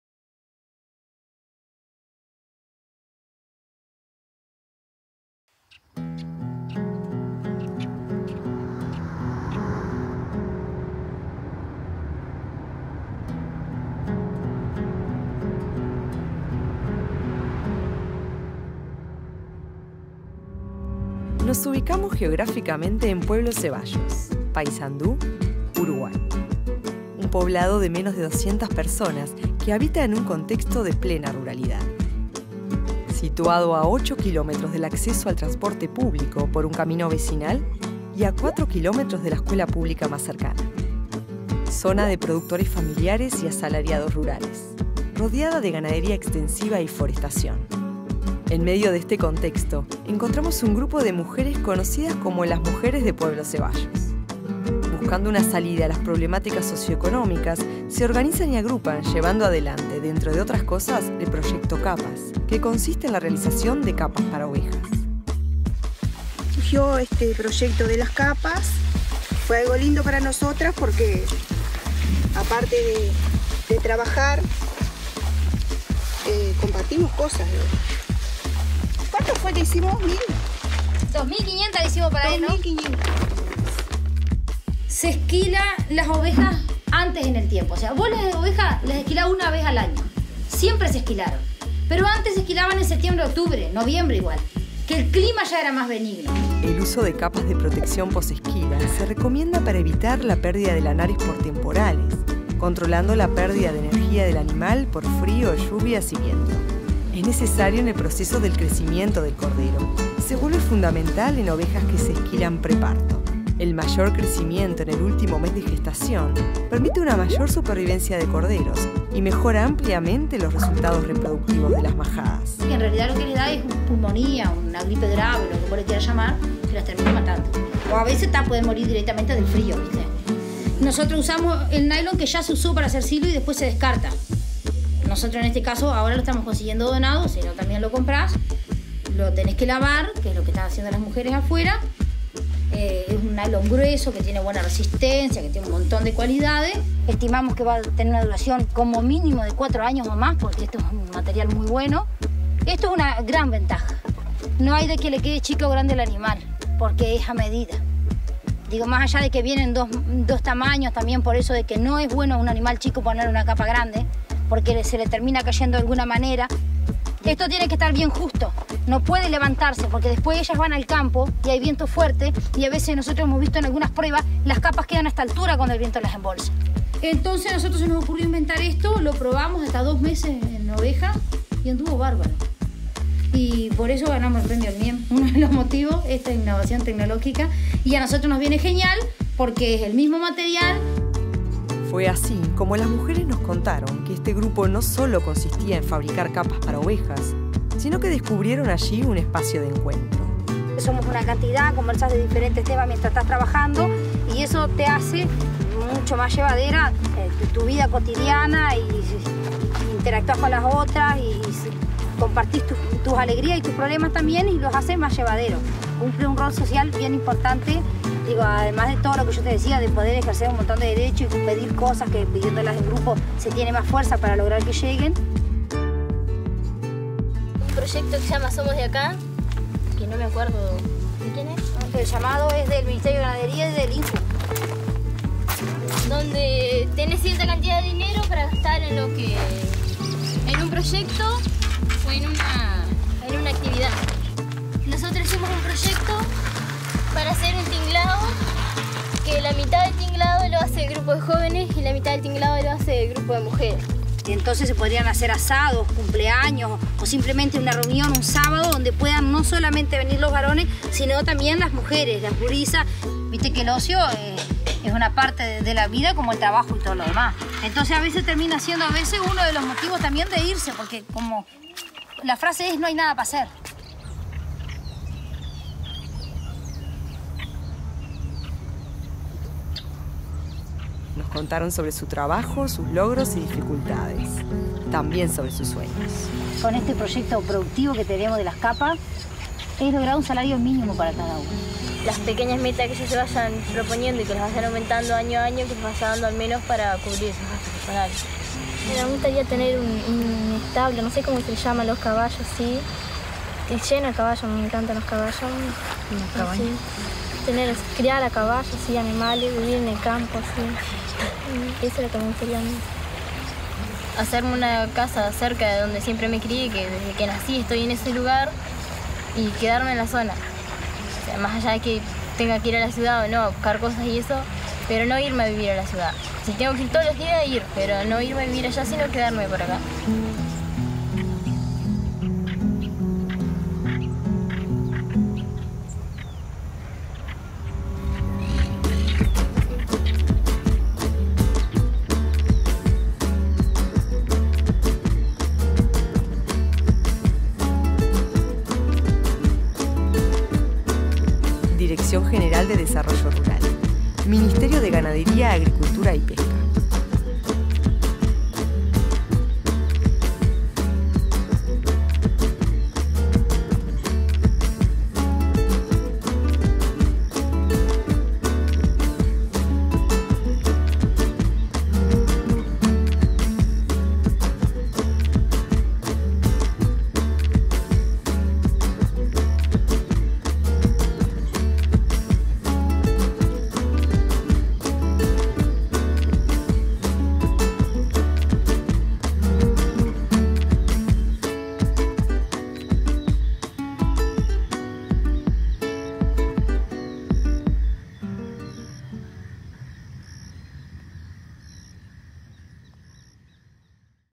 Grupo de Mujeres Rurales de Pueblo Zeballos presentan en Prado 2013 producto innovador para ovinos
Con el apoyo de la Dirección General de Desarrollo Rural, un grupo de mujeres rurales de Pueblo Zeballos (Paysandú), se presentan en stand de MGAP en Expo Prado 2013. Ante una nutrida concurrencia, tendrán la ocasión de mostrar las capas para ovinos recién nacidos que producen, al tiempo que hablarán sobre su innovador proceso de desarrollo rural que vienen celebrando.